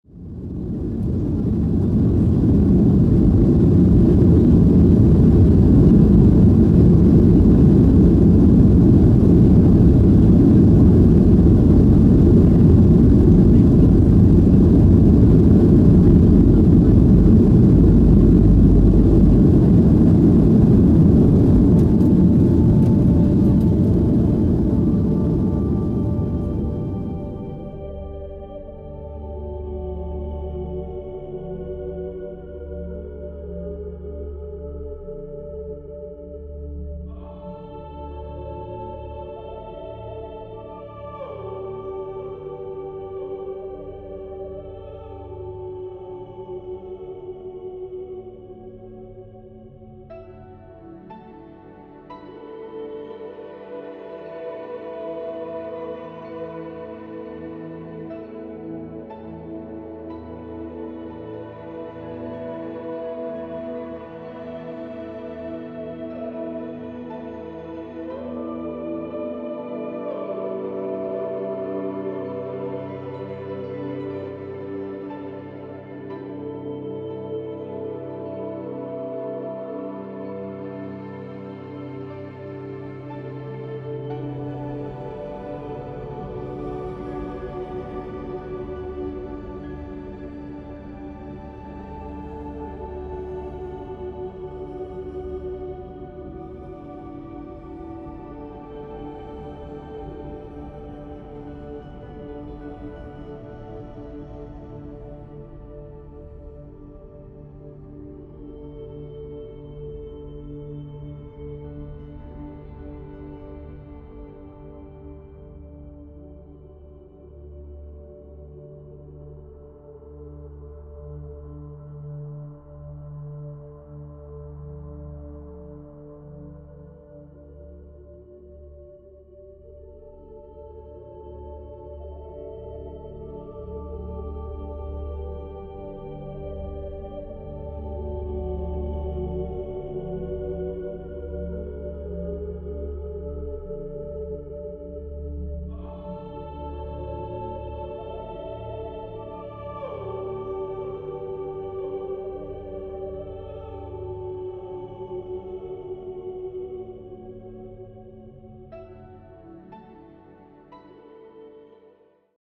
Start samolotu. Hurghada. Egipt. Lotnisko w Hurghadzie.Take-Off from Hurghada. Egypt airport.
takeoff landing plane Podróże po świecie. Wakacje w Egipcie. Wycieczka do Egiptu. Pobyt w hotelu w Hurghadzie.